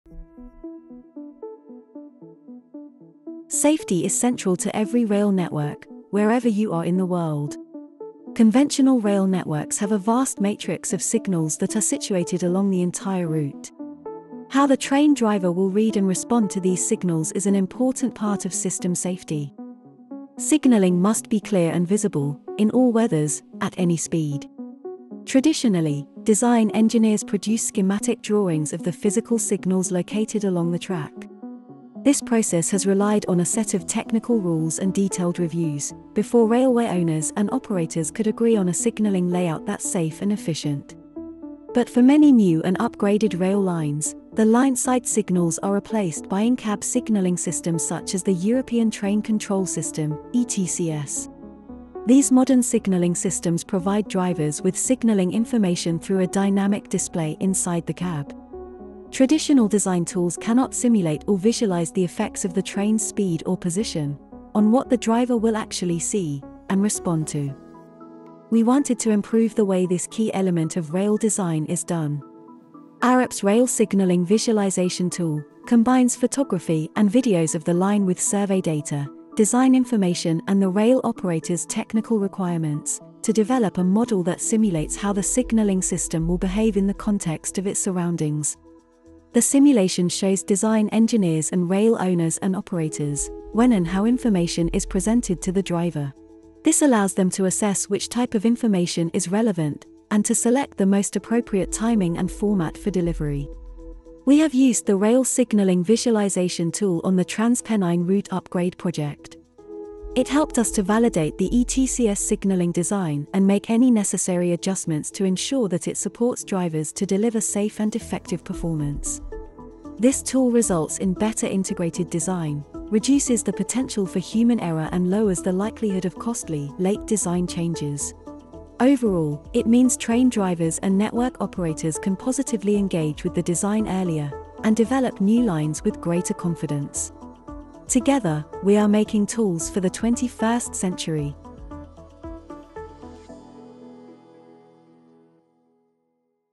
Arup's Digital Solutions: Rail Signalling Visualisation Tool
The Rail Signalling Visualisation Tool (RSVT) we developed, allows design engineers to assess how drivers see and respond to in-cab signalling information to ensure that they receive the right information, in the right format, at the right time.

Design engineers and railway owners and operators can use the RSVT to evaluate at an early stage of the design process how the signalling system will behave in the context of its surroundings. The RSVT enables better design and evaluation of rail signalling systems, improving operational safety and making the design and review process more efficient.